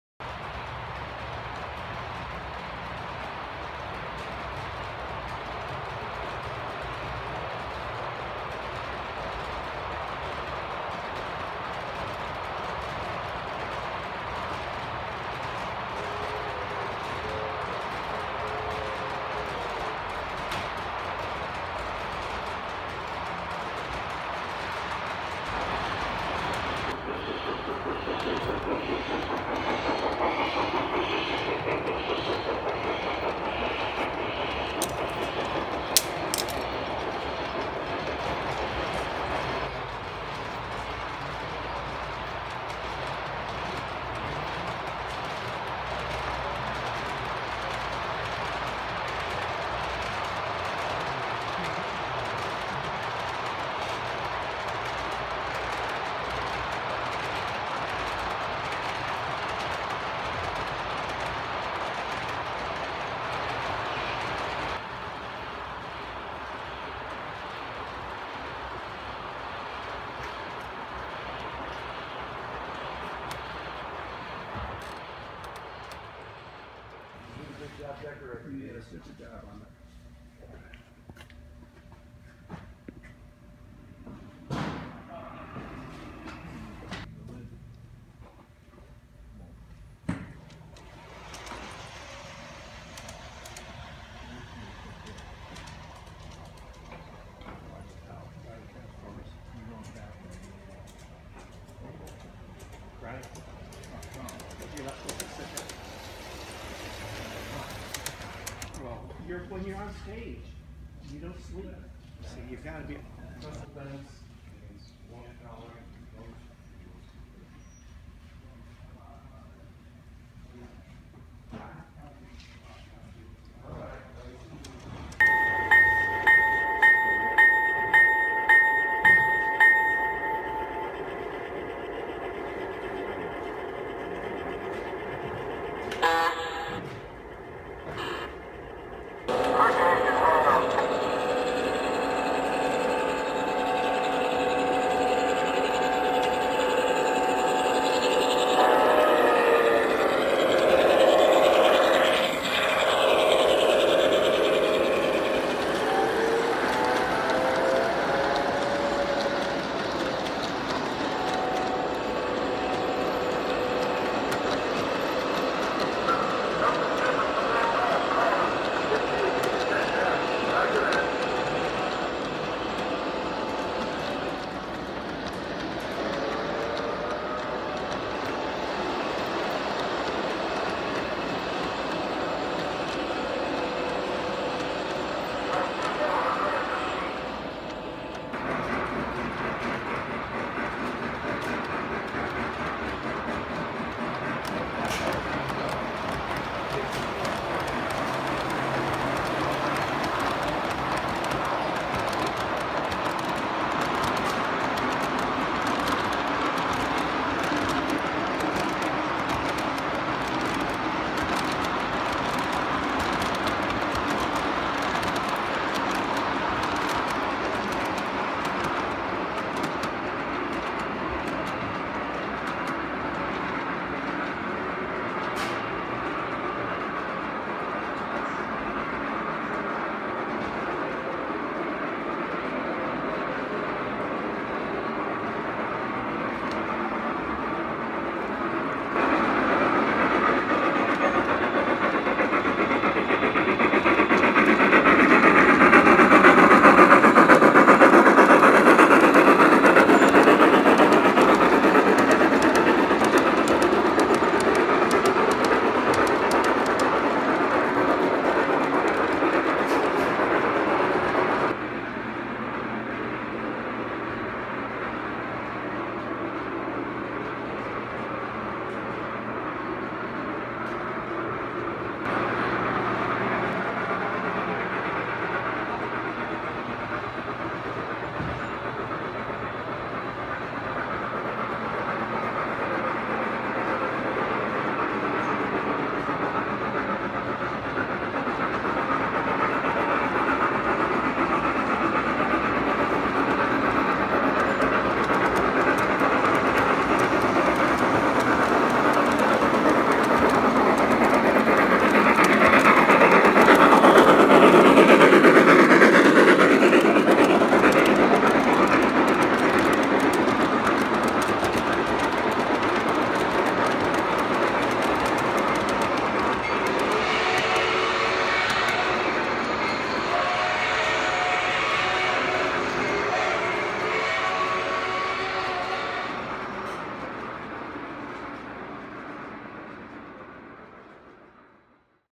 Set up Final
Winter Fest starts tomorrow.

The final touches are made, barricades are up, we're as ready as we'll ever be. "The train is about to leave the station. ALL ABOARD".